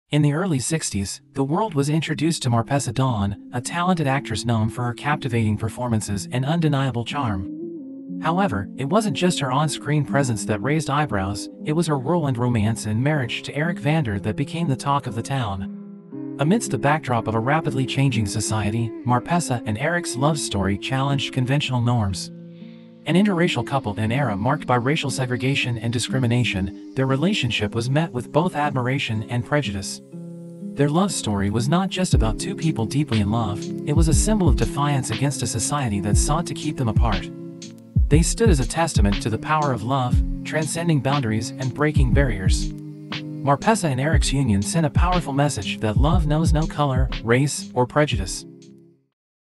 Marpessa Dawn and Eric Vander: A Love That Defied Expectations ❤️📽️
In the early '60s, the world was introduced to Marpessa Dawn, a talented actress known for her captivating performances and undeniable charm. However, it wasn't just her on-screen presence that raised eyebrows; it was her whirlwind romance and marriage to Eric Vander that became the talk of the town.

Amidst the backdrop of a rapidly changing society, Marpessa and Eric's love story challenged conventional norms. An interracial couple in an era marked by racial segregation and discrimination, their relationship was met with both admiration and prejudice.

Their love story was not just about two people deeply in love; it was a symbol of defiance against a society that sought to keep them apart. They stood as a testament to the power of love, transcending boundaries and breaking barriers. Marpessa and Eric's union sent a powerful message that love knows no color, race, or prejudice.

Their story is a reminder that true love can conquer even the most formidable challenges. It's a testament to the resilience of the human spirit and the enduring power of love to overcome societal expectations and prejudice. Marpessa and Eric's legacy continues to inspire conversations about love, equality, and the enduring pursuit of happiness. ❤️🌟